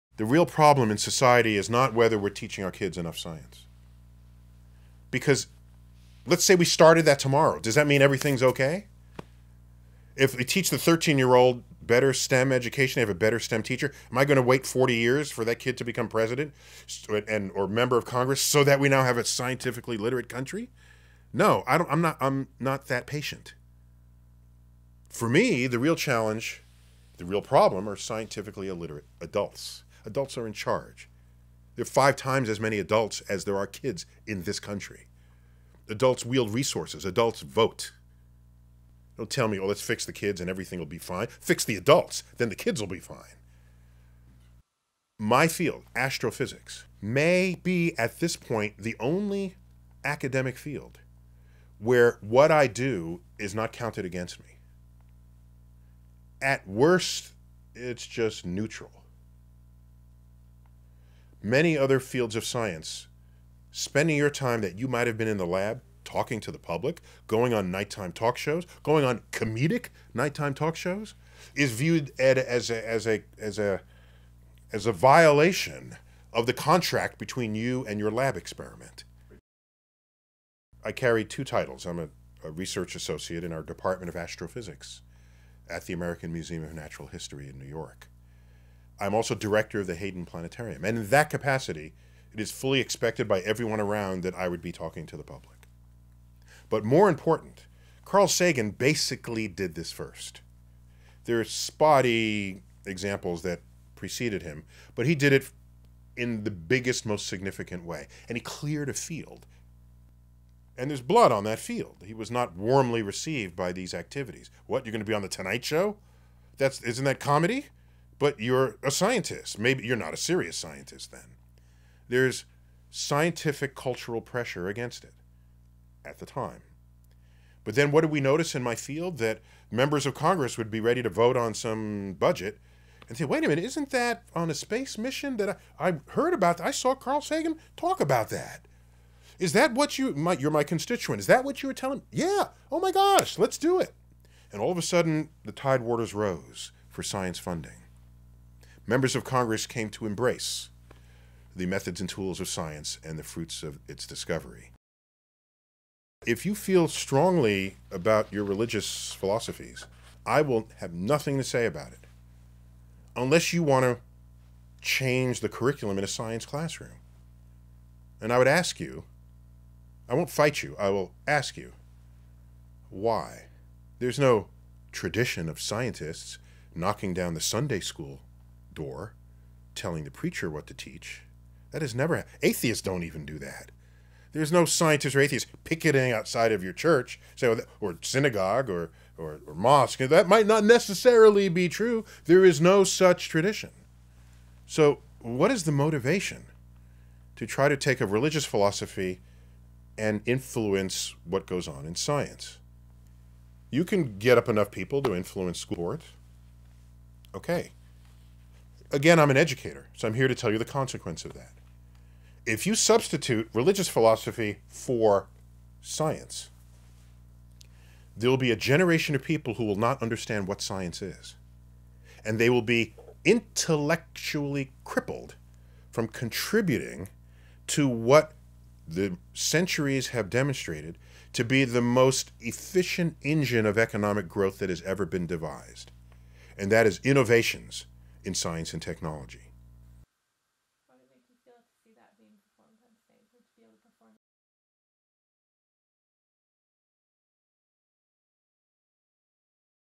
Neil deGrasse Tyson fala sobre criacionismo, divulgação científica e ensino da ciência
Pela primeira vez na sua carreira, o Astrofísico critica a posição dos Criacionistas que tentam modificar o currículo de escolas para incluir a sua visão não científica.
Além de falar sobre a importância do ensino da ciência, ele também relata as críticas dirigidas aos pesquisadores que se dedicam à divulgação científica.